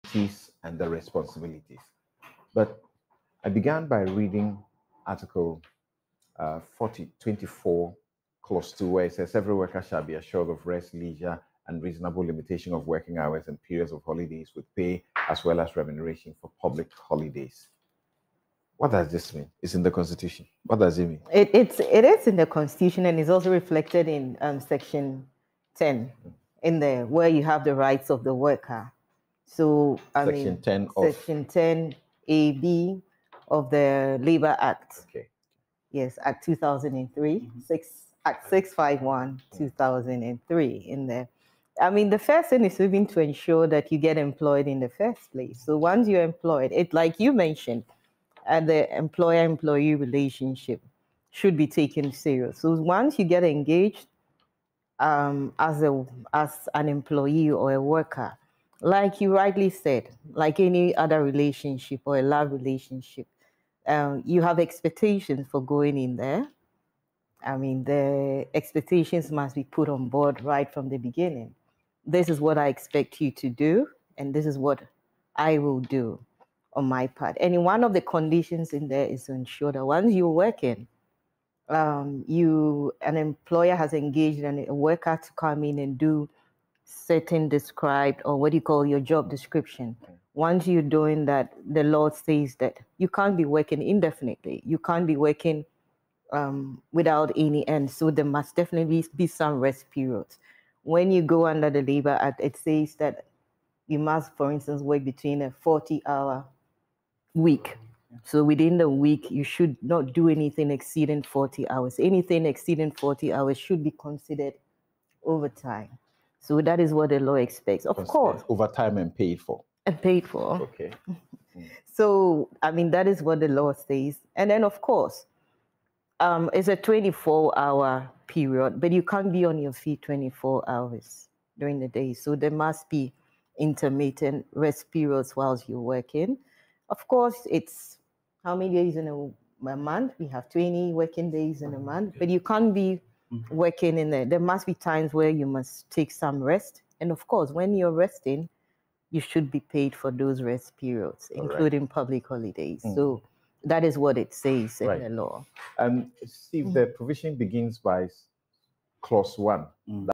Working beyond 40 hours a week is considered overtime and must be paid - Ekua Eguakun
The Managing Partner of KEDE LAW, Ekua Eguakun, has stated that an employee should not be made to work beyond 40 hours a week, according to the law.